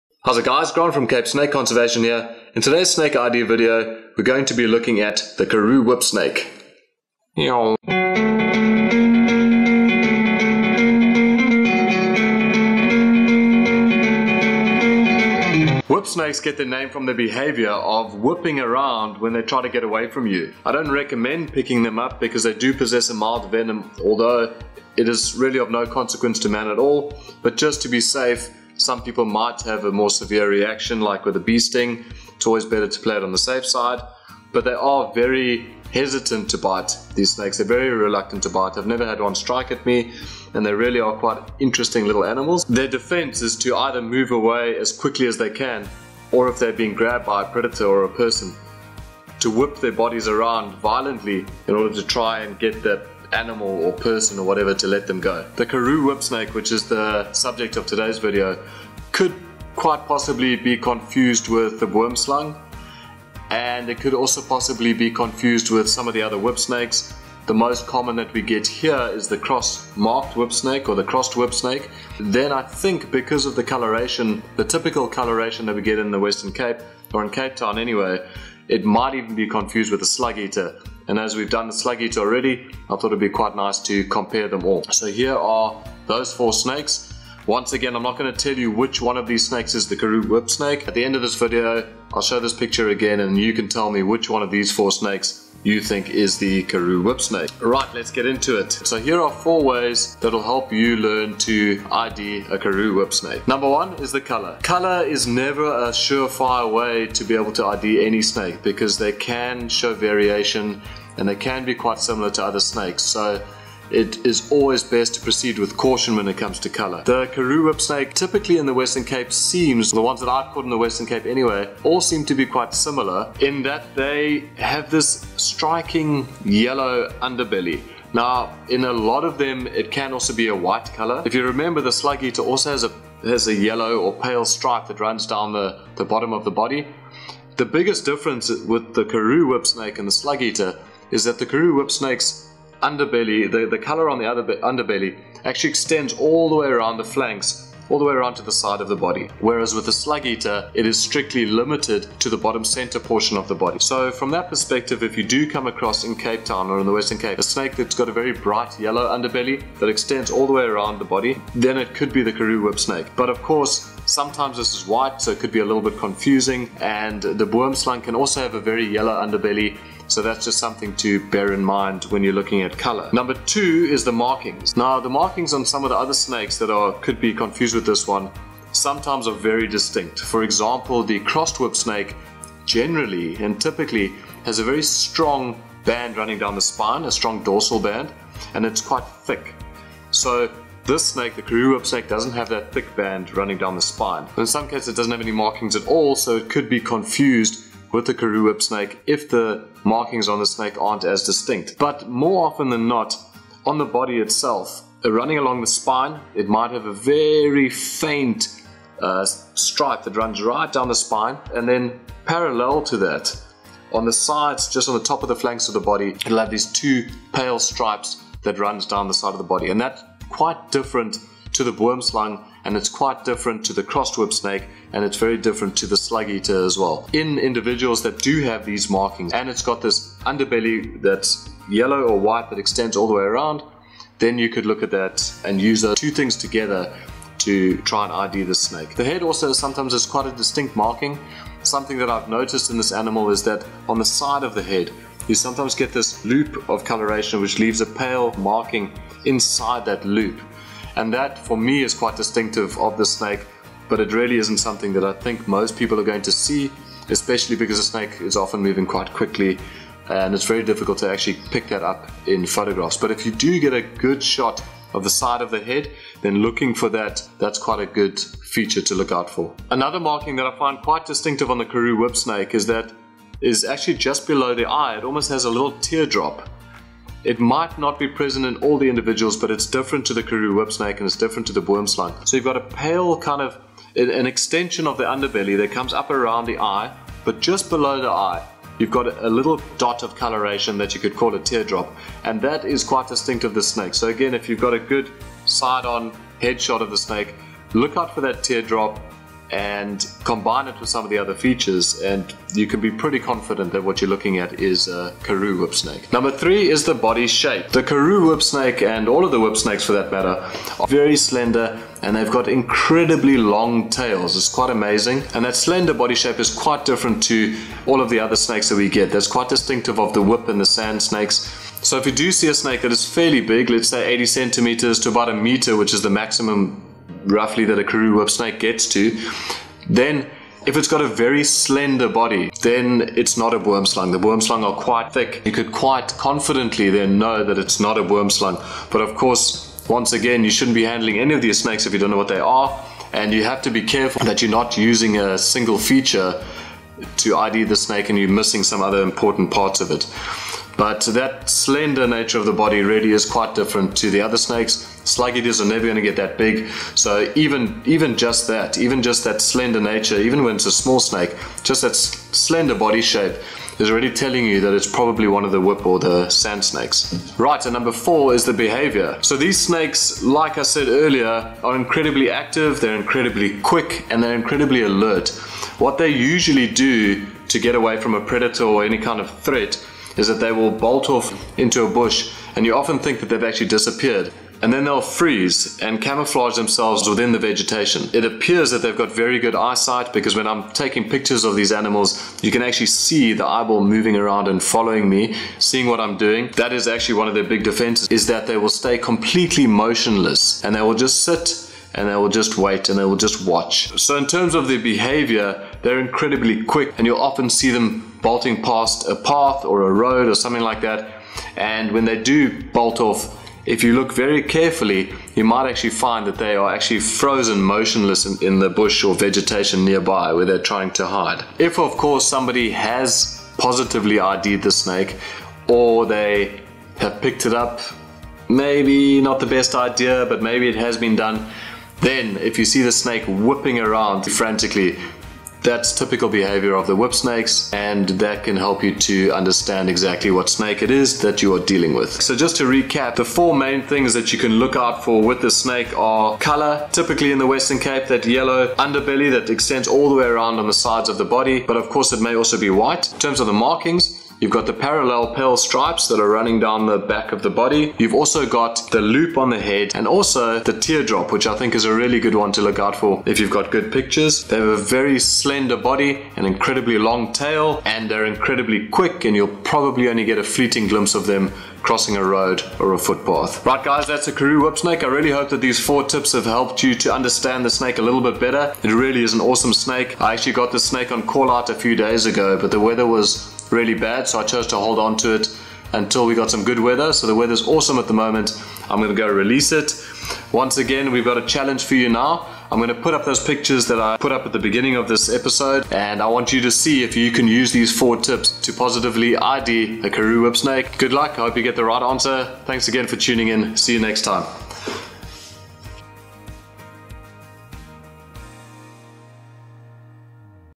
Identify a Karoo Whip Snake in 4 Easy Steps
The Karoo whip snake is an alert and active snake. They get their name from their defensive thrashing behaviour, used when something grabs them.

The Karoo whip snake is fast and will speed away given the opportunity. They do possess a mild venom but it's not medically significant and they are very reluctant to bite.

Learn how to identify a Karoo whip snake with ease using my 4 tips in this video.